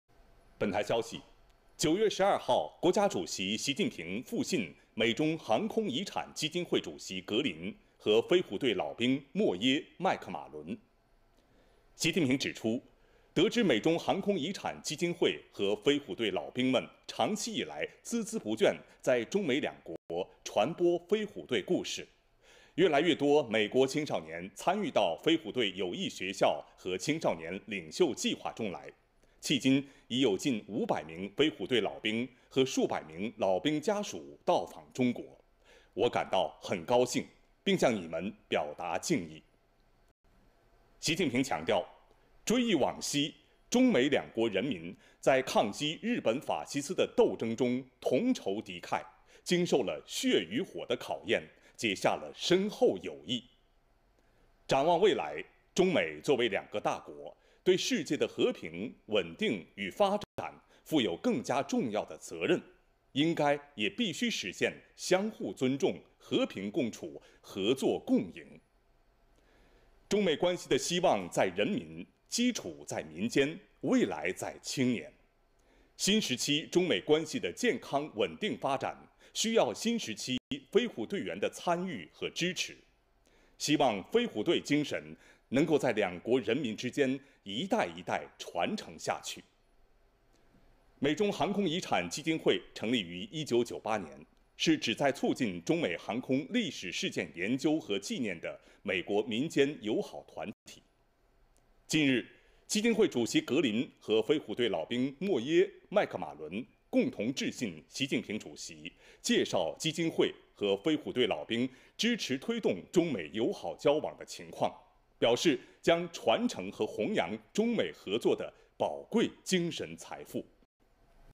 习近平复信美中航空遗产基金会主席和飞虎队老兵
9月12日，国家主席习近平复信美中航空遗产基金会主席格林和飞虎队老兵莫耶、麦克马伦。

习近平指出，得知美中航空遗产基金会和飞虎队老兵们长期以来孜孜不倦在中美两国传播飞虎队故事，越来越多美国青少年参与到“飞虎队友谊学校和青少年领袖计划”中来，迄今已有近500名飞虎队老兵和数百名老兵家属到访中国，我感到很高兴，并向你们表达敬意。

习近平强调，追忆往昔，中美两国人民在抗击日本法西斯的斗争中同仇敌忾，经受了血与火的考验，结下了深厚友谊。展望未来，中美作为两个大国，对世界的和平、稳定与发展负有更加重要的责任，应该也必须实现相互尊重、和平共处、合作共赢。中美关系的希望在人民，基础在民间，未来在青年。新时期中美关系的健康稳定发展，需要新时期飞虎队员的参与和支持，希望飞虎队精神能够在两国人民之间一代一代传承下去。

美中航空遗产基金会成立于1998年，是旨在促进中美航空历史事件研究和纪念的美国民间友好团体。近日，基金会主席格林和飞虎队老兵莫耶、麦克马伦共同致信习近平主席，介绍基金会和飞虎队老兵支持推动中美友好交往的情况，表示将传承和弘扬中美合作的宝贵精神财富。